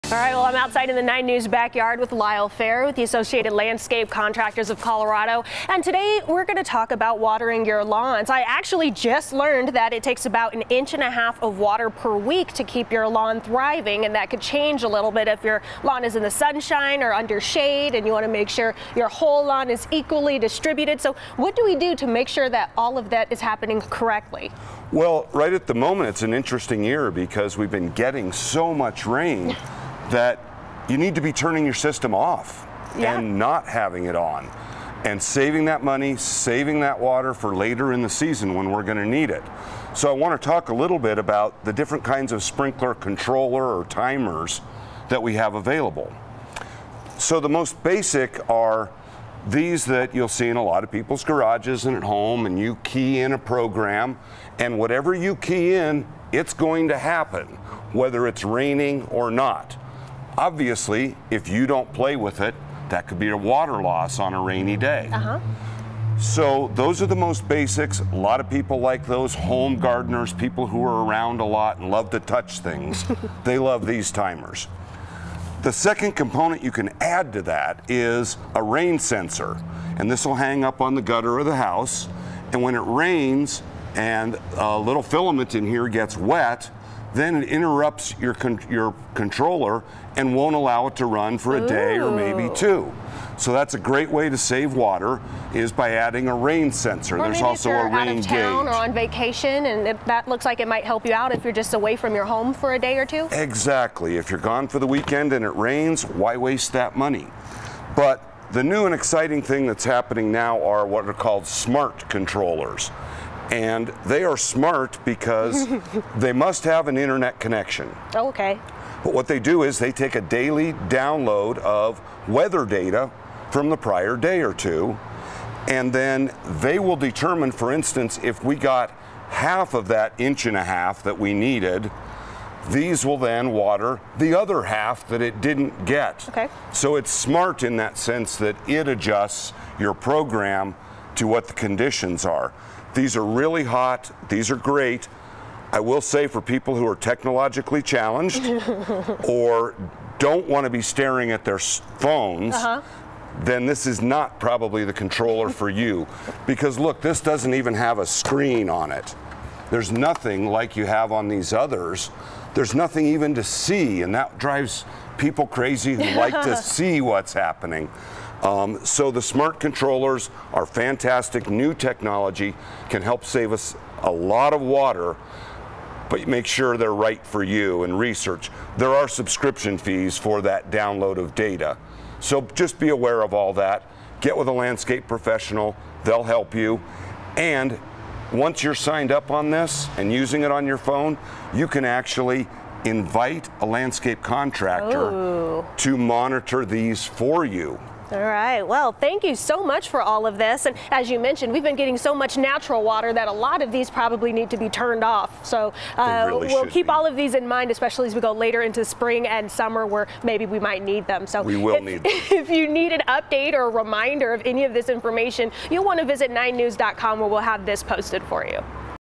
How you can keep you lawn evenly irrigated
Lyle Fair with the Associated Landscape Contractors of Colorado joins us to talk about how you can water evenly.

9NEWS (KUSA) is located in Denver, Colorado.